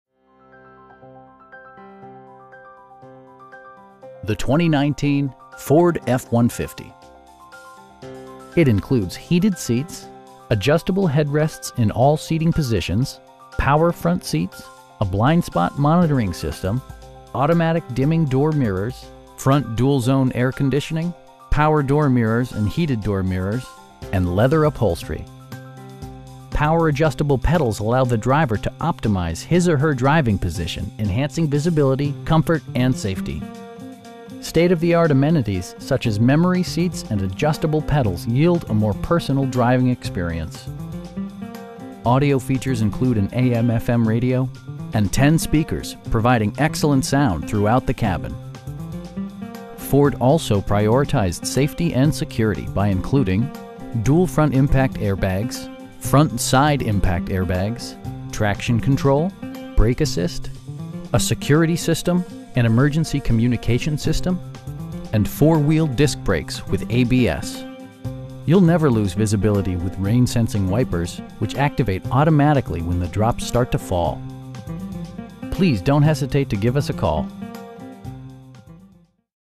2019 Ford F-150 Lariat in Madison, TN 37115
Town & Country
101 Anderson Lane

The 2019 Ford F-150. It includes heated seats, adjustable headrests in all seating positions, power front seats, a blind spot monitoring system, automatic dimming door mirrors, front dual-zone air conditioning, power door mirrors and heated door mirrors, and leather upholstery. Power adjustable pedals allow the driver to optimize his or her driving position, enhancing visibility, comfort and safety. State-of-the-art amenities such as memory seats and adjustable pedals yield a more personal driving experience. Audio features include an AM/FM radio, steering wheel mounted audio controls, and 10 speakers, providing excellent sound throughout the cabin. Ford also prioritized safety and security by including: dual front impact airbags, front side impact airbags, traction control, brake assist, a security system, an emergency communication system, and 4 wheel disc brakes with ABS. You'll never lose visibility with rain sensing wipers, which activate automatically when the drops start to fall. You will have a pleasant shopping experience that is fun, informative, and never high pressured. Please don't hesitate to give us a call.